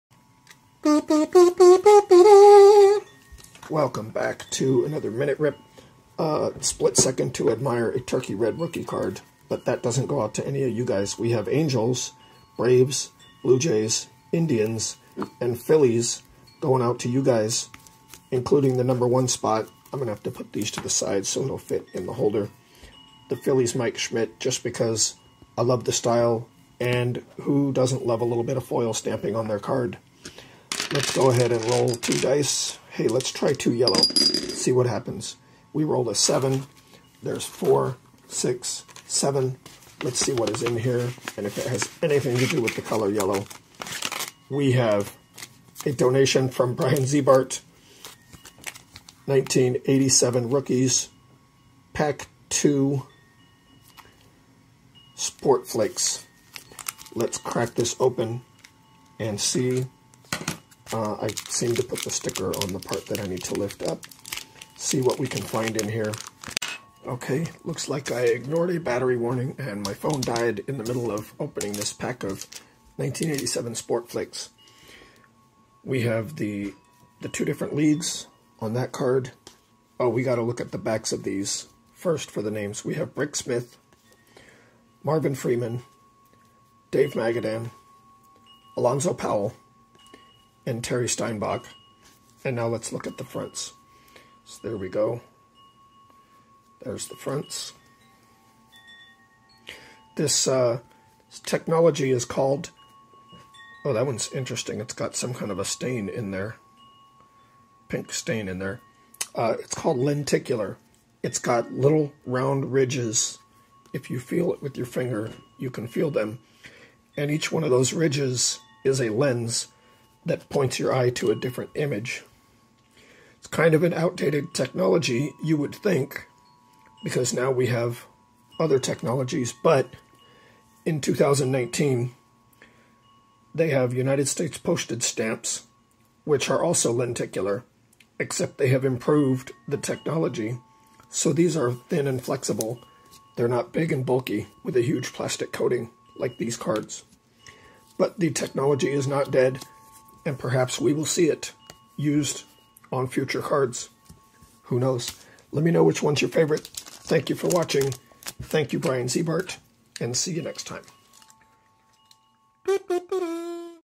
Minute Rip 012522
Check out these videos to see the generous  donations we've received for Minute Rips from...

A series of quick videos where I take about one minute to rip open a pack of  baseball cards.